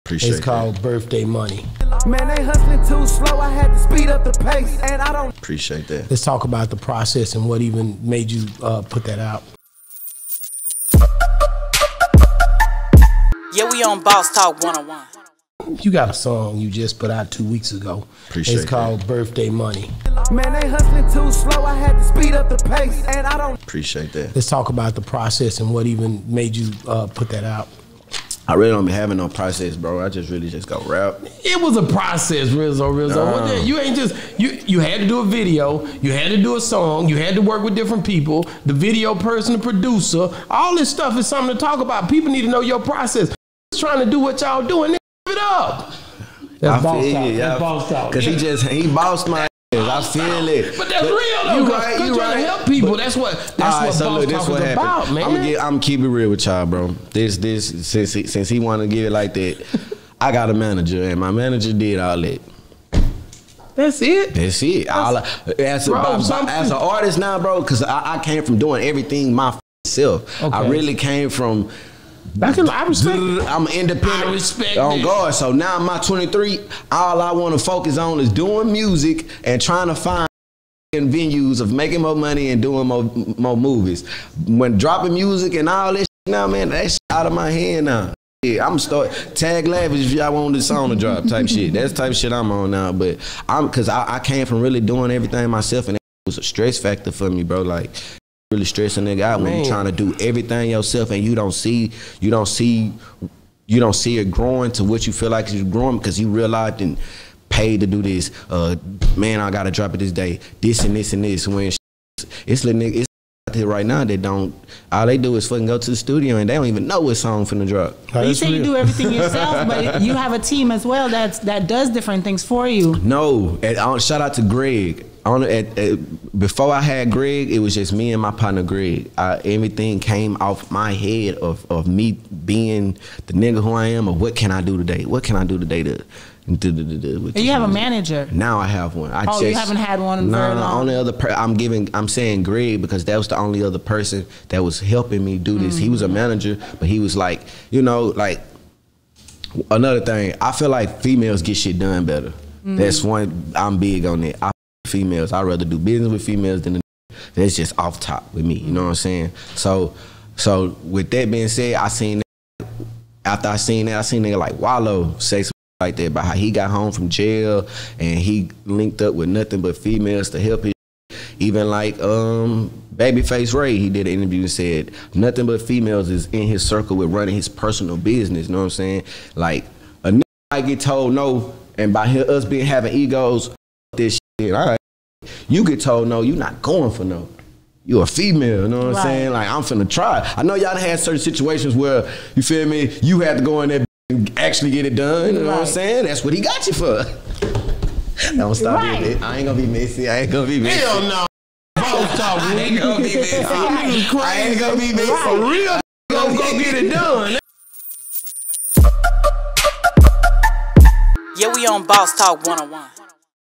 Rizzoo Rizzoo on Birthday Money The Process Having to do the Work Myself | Wallo Trust only Women?
Rizzoo Rizzoo on Birthday Money The Process Having to do the Work  Myself | Wallo Trust Women